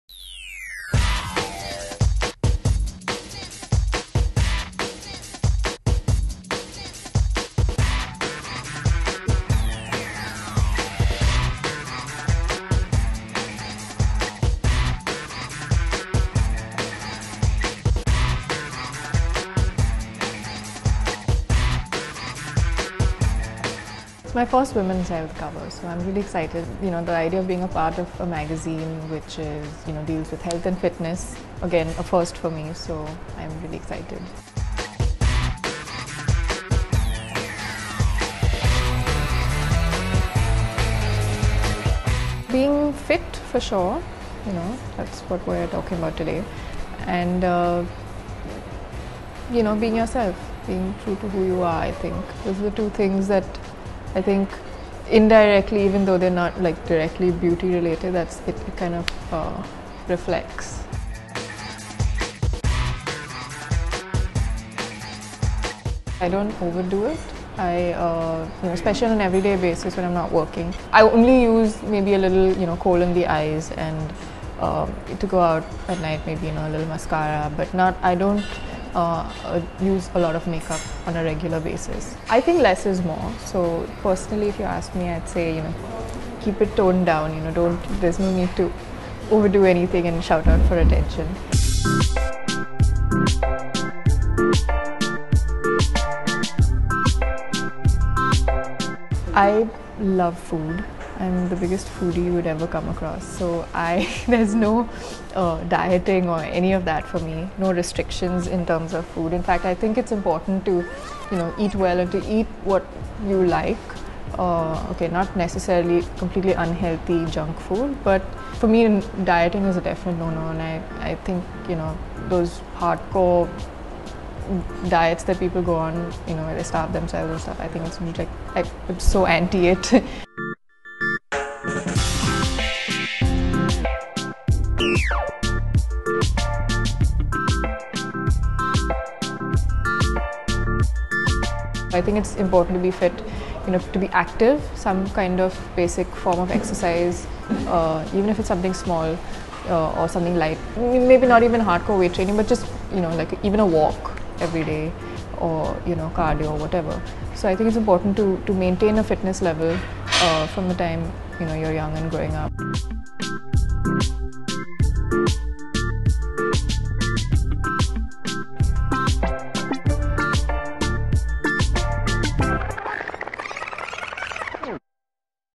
Diana Penty turns cover girl for Women's Health magazine
Cocktail girl Diana Penty shoots for the cover of Women's Health magazine and speaks on how she manages to remain fit, her career in Bollywood and the type of person that she is.